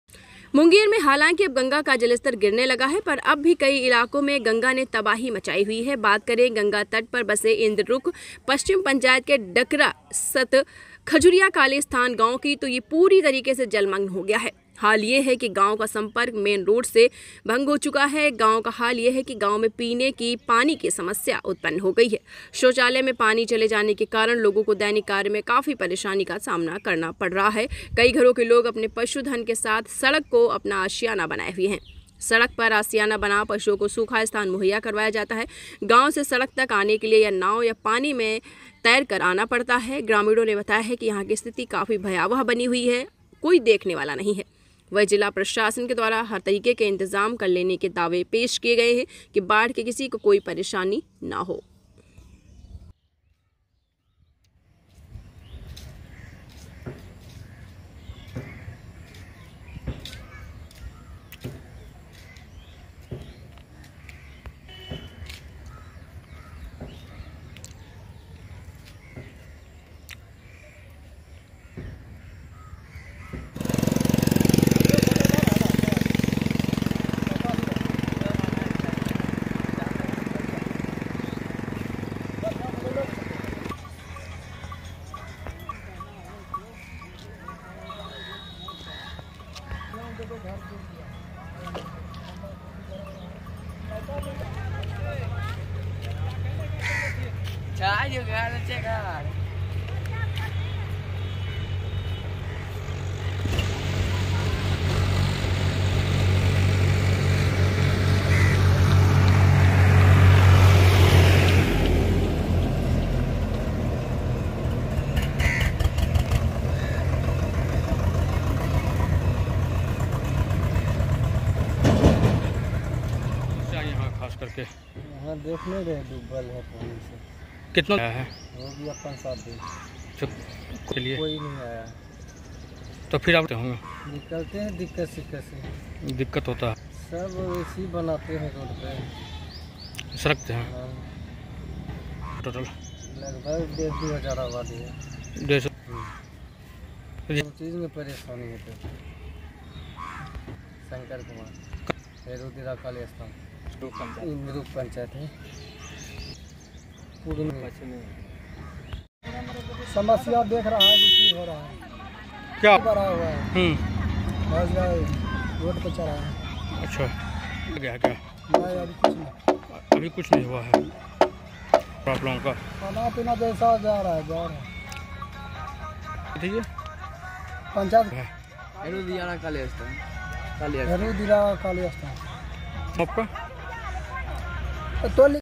मुंगेर में बाढ़ के पानी में डूबा यह गांव, सड़क किनारे रहने को मजबूर हुए लोग
मुंगेर लाइव में आप का स्वागत है। इसी तरह के हर छोटी बड़ी अपडेट पाने के लिए बने रहे मुंगेर लाइव के साथ। 🙏🙏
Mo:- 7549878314